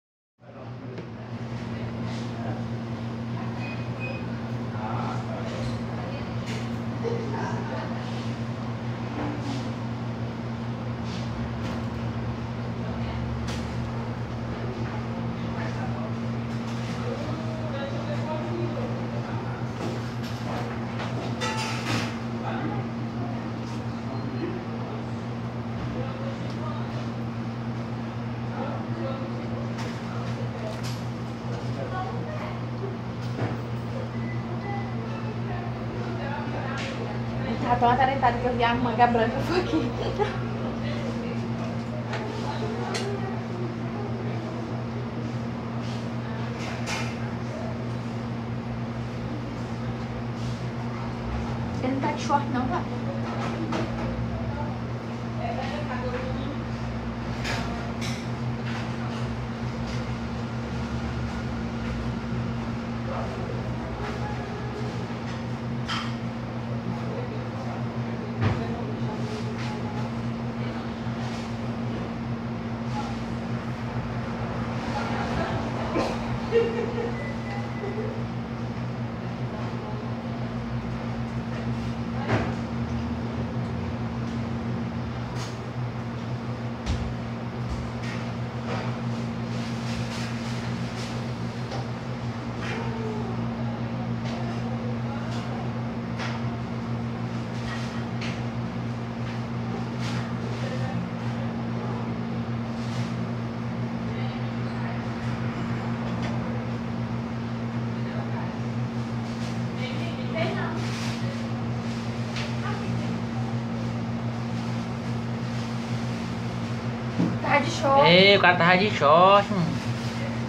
Momentos tenso durante assalto a uma padaria na noite deste sábado em Araguaína
Na noite deste sábado (13), dois elementos chegaram em uma moto e anunciaram um assalto a uma padaria, que fica localizada na Avenida Santos Dumont, em Araguaína.